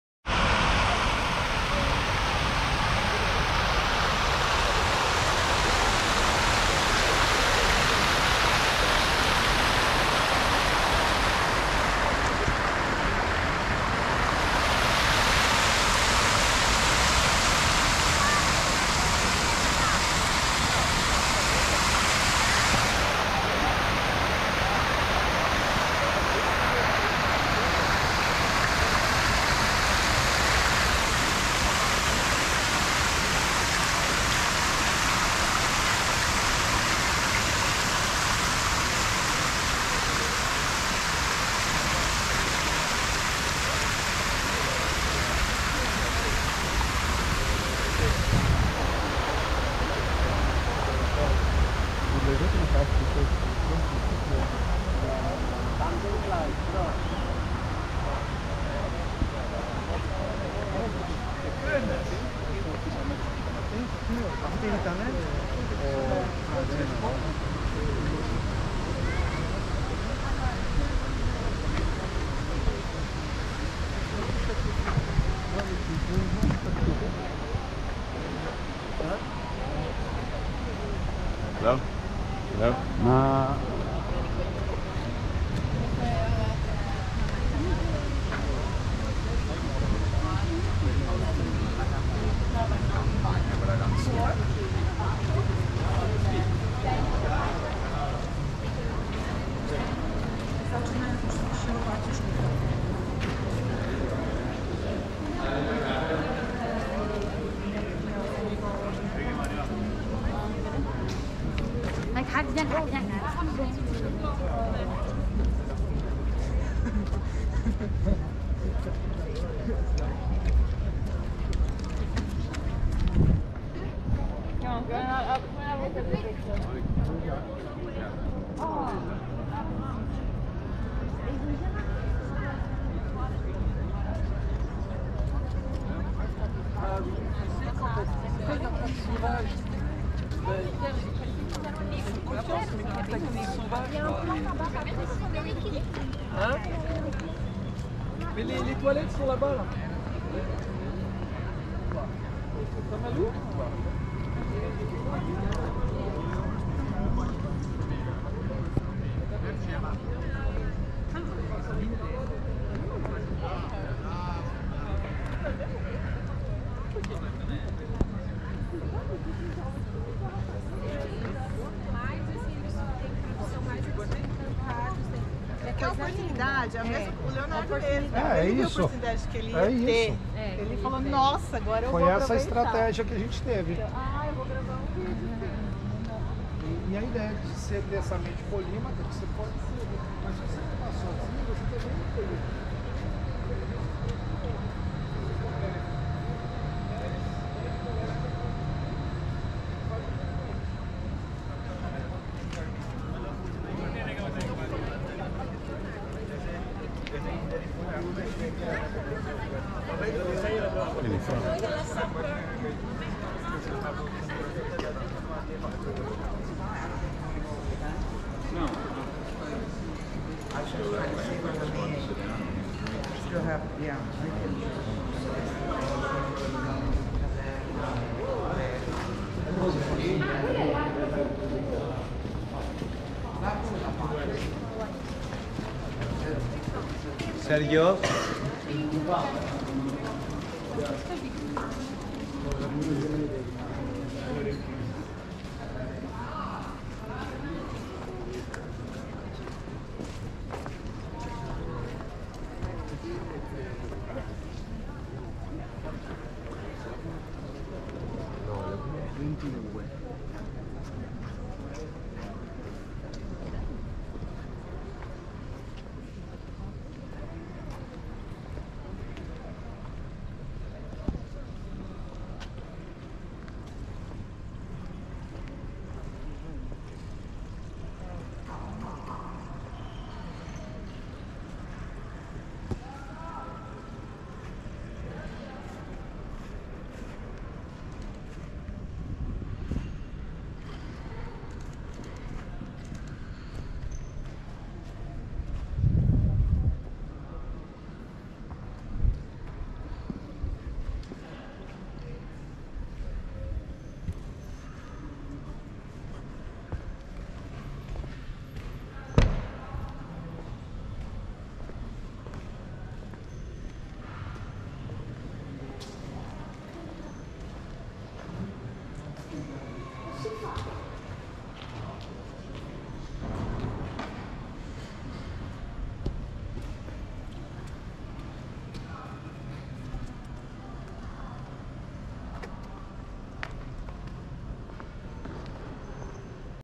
Milan City Walk - the Stunning Sforza Castle 4K
This walk takes in the impressive Castello Sforza in Milan. This impressive place was the home to the Dukes of Milan,. See the beautiful Architecture and courtyards :)